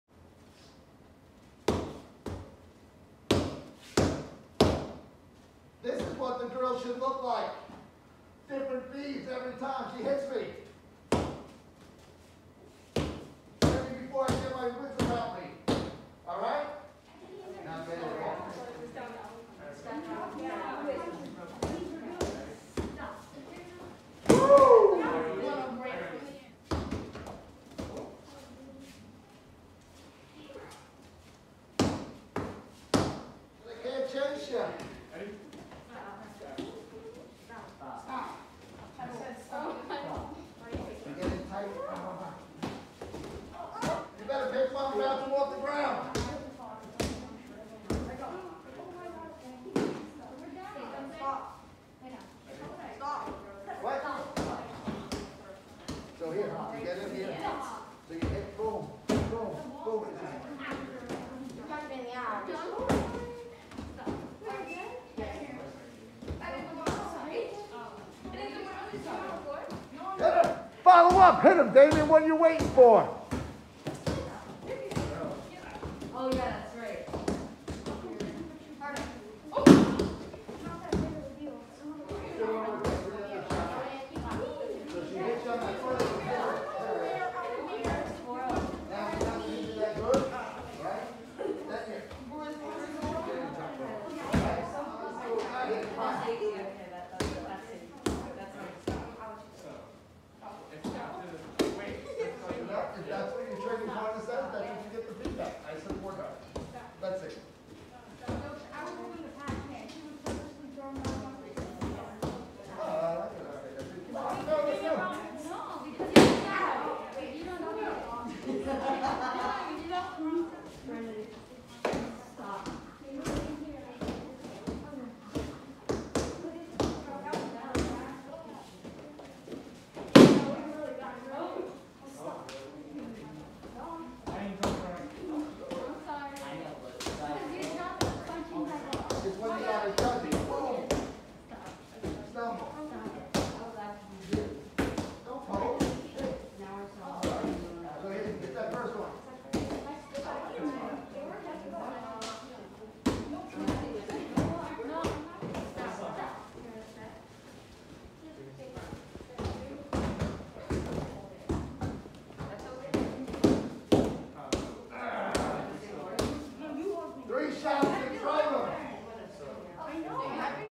Fence Drill Add On
Here is A deill from last night class adding on to the Fence Drill. Here we added take downs and punches back to the possible feeds and outcomes.
But really loved my New students in this drill the where doing great.